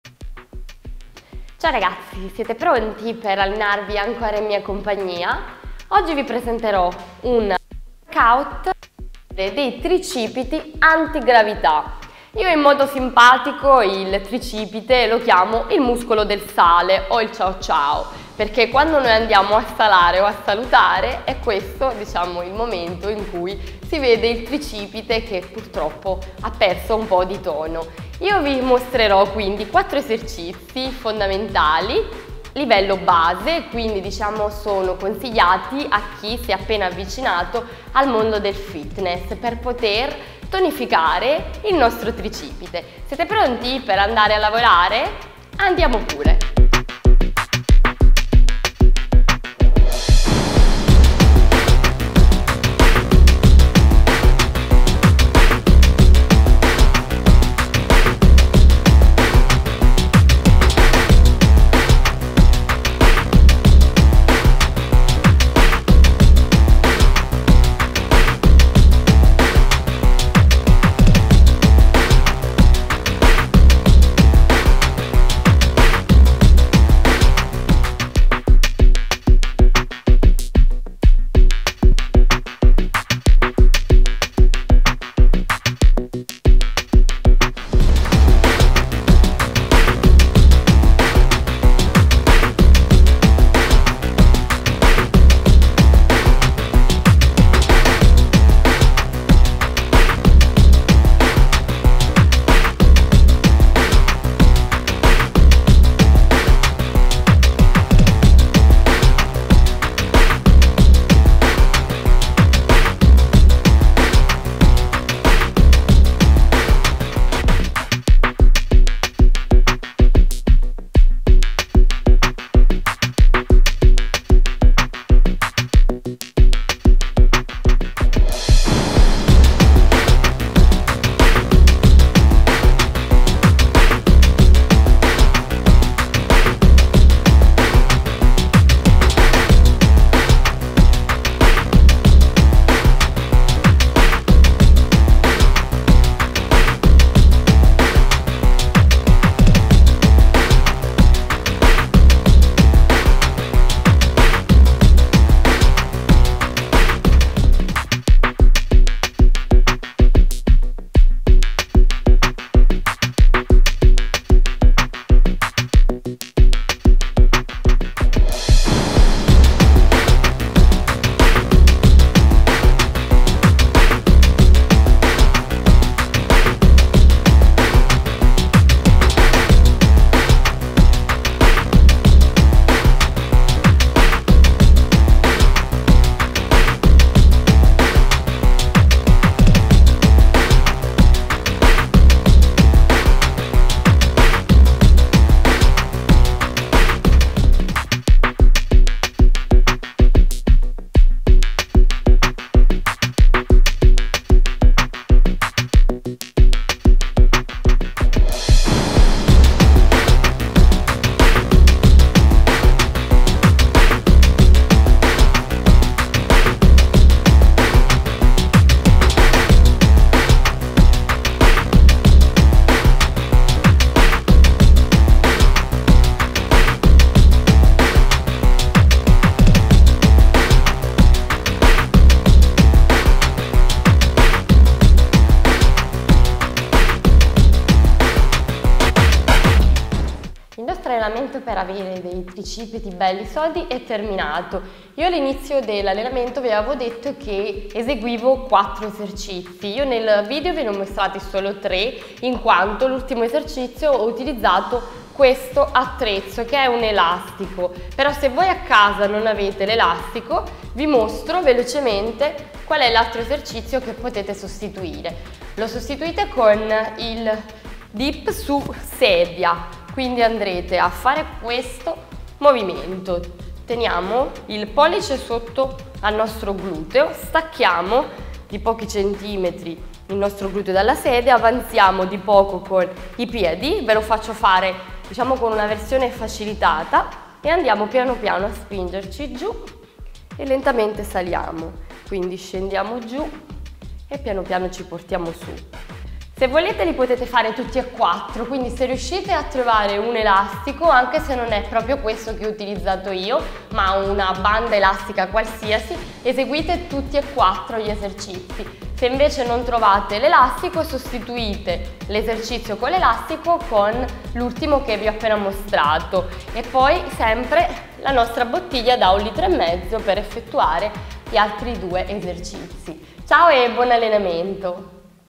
Allenamento Tricipiti Sodi - Livello 1 Principiante Molto Facile
Il trascorrere del tempo mette a dura prova una piccola parte del corpo di ogni donna, il tricipite del braccio. Questo allenamento costituito da pochi e semplici esercizi vi permetterà di dire addio alle braccia cadenti. Il segreto per lavorare con efficacia sulla zona del tricipite è quello di mantenere fisso il braccio al corpo e non muovere mai il gomito avanti e dietro; l'unica parte che si deve flettere ed estendere è l'avambraccio. Si consiglia di eseguire 3 /4 round a giorni alterni. Al termine della seduta di allenamento eseguire sempre qualche minuto di stretching.

Note:

Attrezzatura: manubri, elastico, sedia

3 esercizi +1 di bonus (1 round)
30'' di lavoro 10'' di pausa oppure 20 ripetizioni in TOT
40'' di pausa tra un round e l'altro
Si consiglia di eseguire 3 /4 round a giorni alterni.